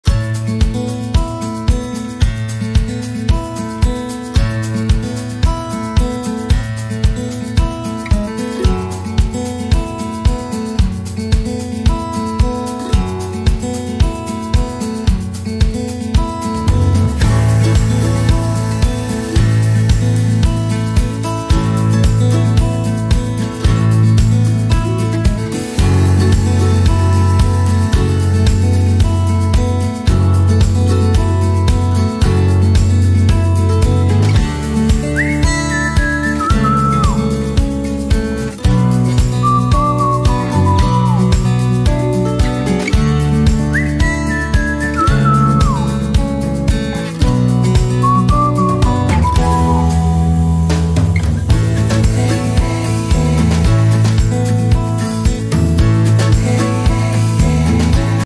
2024 Holy Week at St. Barnabas Presbyterian Church in Richardson, Texas
Relive the powerful moments of Holy Week at St. Barnabas! ✝️
We've compiled a special video capturing the spirit of reflection, worship, and community during this sacred time.
Join us as we revisit Palm Sunday, Maundy Thursday, Good Friday, and the glorious Easter celebration!